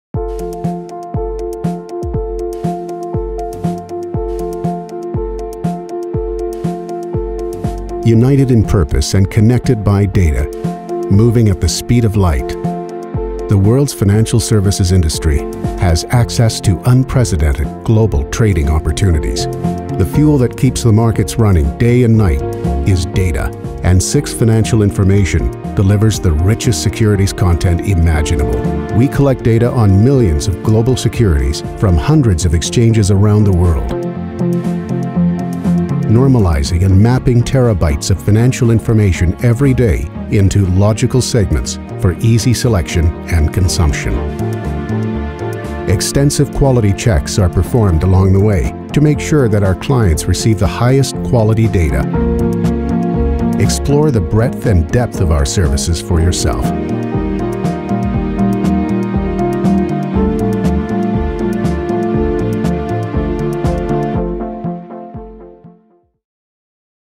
SIX Financial Information: Data
United in purpose and connected by data moving at the speed of light, the world's financial services industry has access to unprecedented global trading opportunities.  The fuel that keeps the markets running day and night is data, and SIX Financial Information delivers the richest securities content imaginable.  We collect data on millions of global securities from hundreds of exchanges around the world, normalizing and mapping terabytes of financial information every day into logical segments for easy selection and consumption.  Extensive quality checks are performed along the way to make sure that our clients receive the highest quality data.  Explore the breadth and depth of our services for yourself.